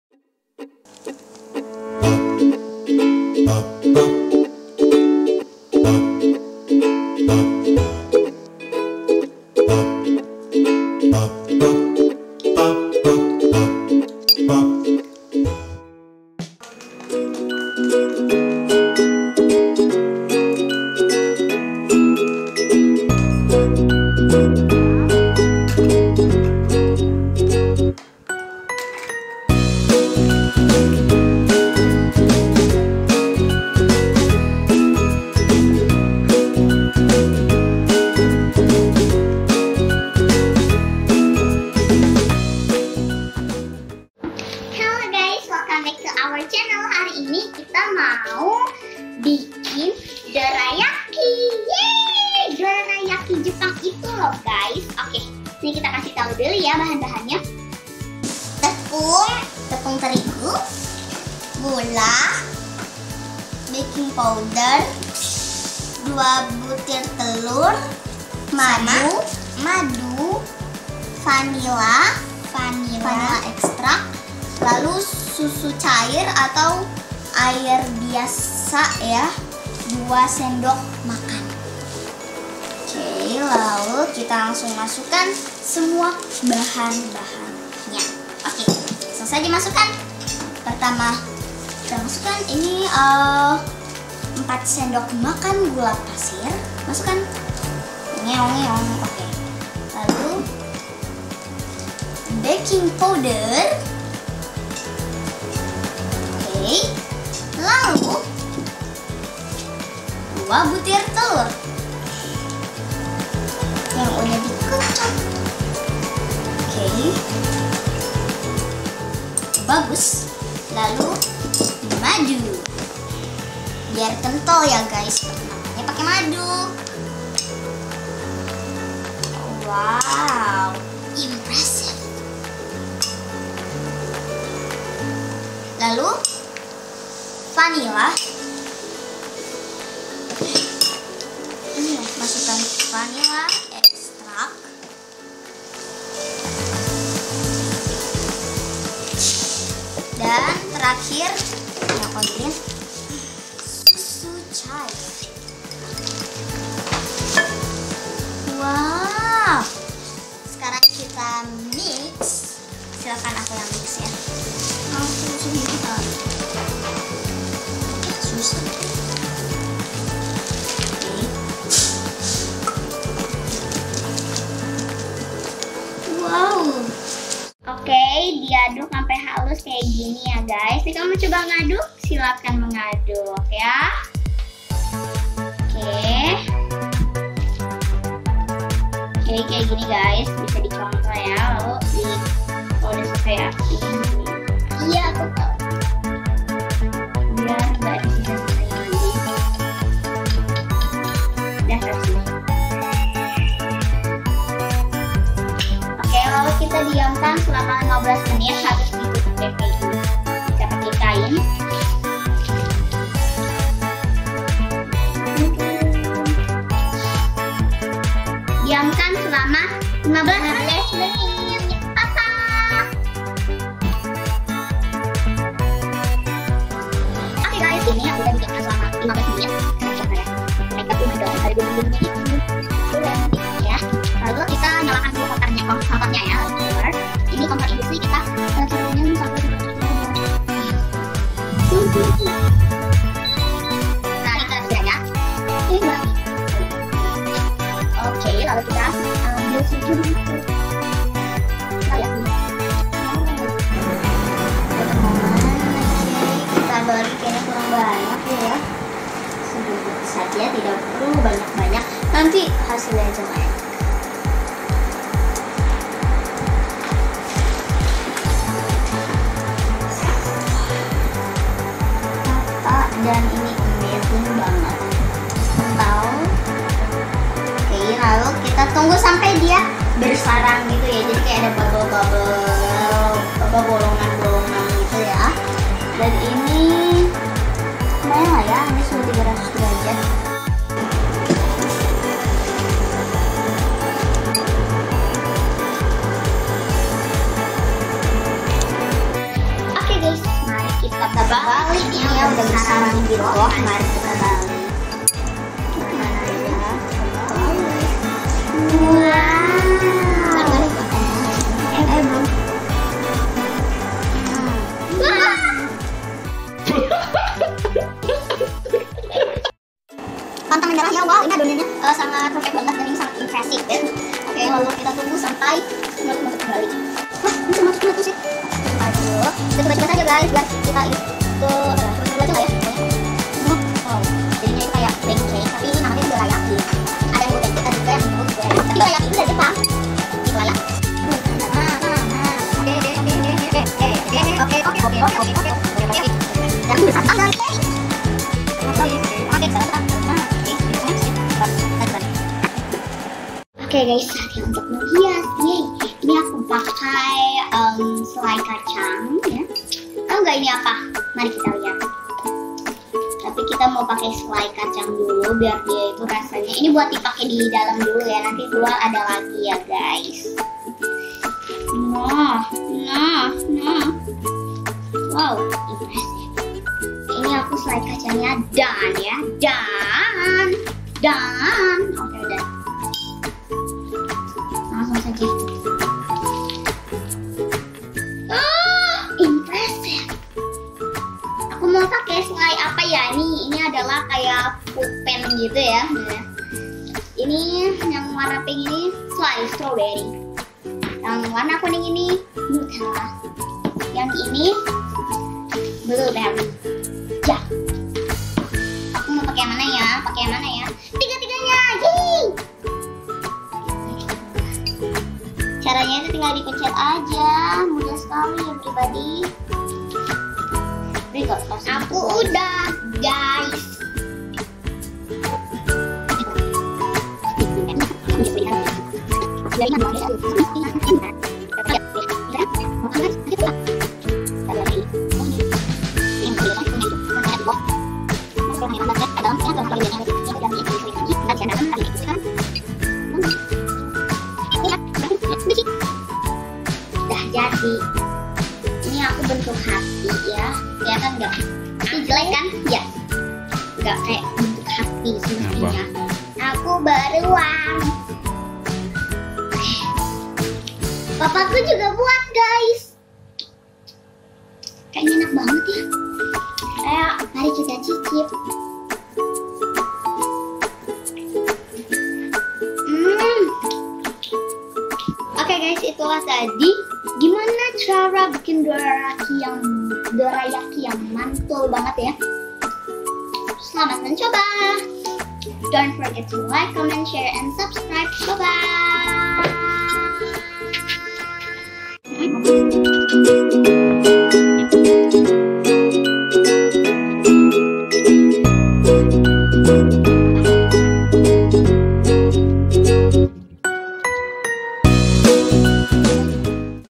Dorayaki Jepang ala K&K
Cemilan kesukaannya Doraemon nih gaess.. resepnya seperti dibawah ini ya, untuk topping bisa bebas suka-suka.. selamat mencoba...🥰

2 butir telur
4 sdm gula
1 sdm madu
1/2 sdt vanilla extract
90 gram tepung terigu
1 sdt baking powder
2 sdm susu cair atau air mineral

topping
- selai kacang
- selai lainnya (coklat, strawberry, blueberry)

source: delmira cooking